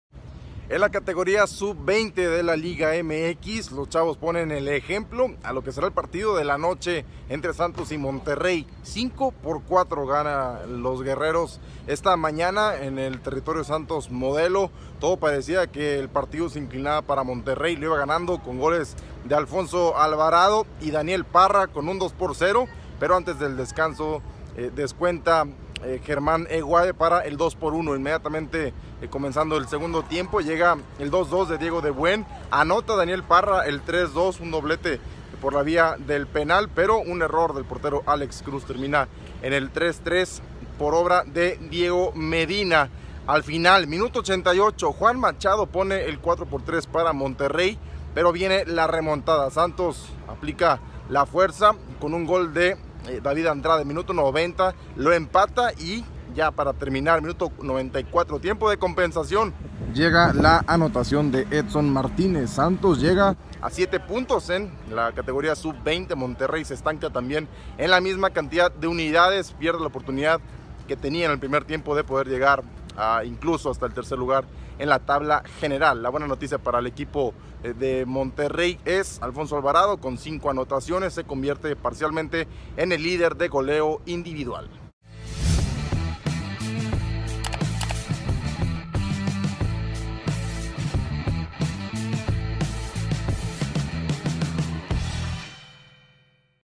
Rayados | Se le escapó el triunfo a los rayaditos | ONCE Diario
Partido vibrante en el TSM en la categoría Sub 20. Santos se repuso de la desventaja y logró una emocionante victoria ante Rayados. El reporte con Adrián Maldonado 📹